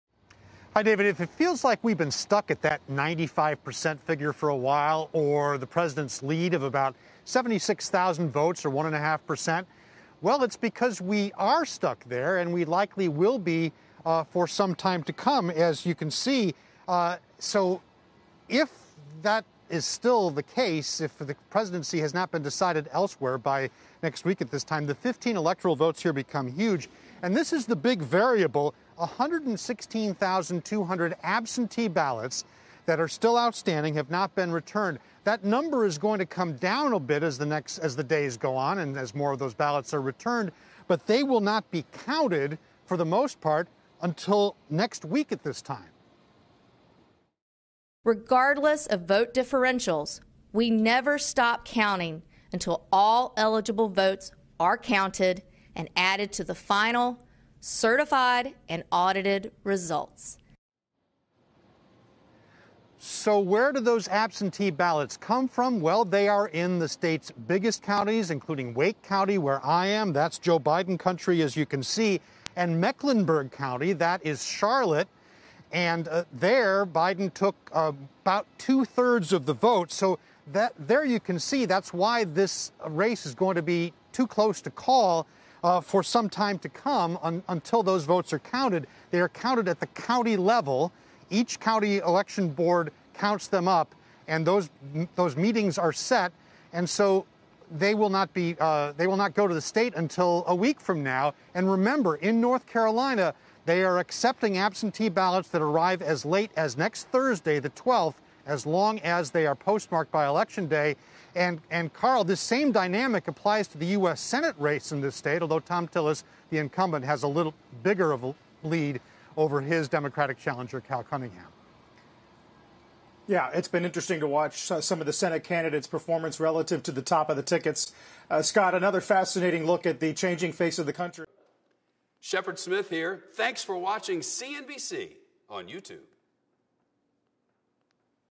Here's where North Carolina's outstanding absentee ballots are from
CNBC's Scott Cohn reports from North Carolina on how the election results are being tallied.